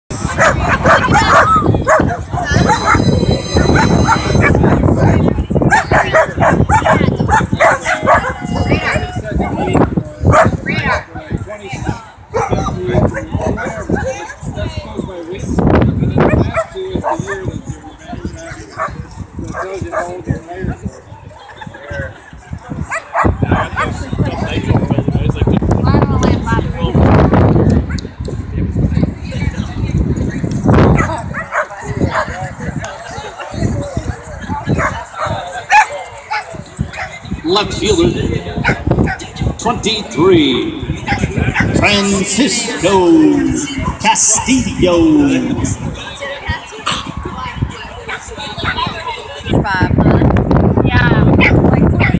Wisconsin Sheltie Rescue at Bark in the Park 2015 scanning around the crowd
Here is a scan of our Wisconsin Sheltie Rescue group at the game. also some towards the end of my family, with my grandpuppies the yellow lab, the white bichon mix and the two corgis!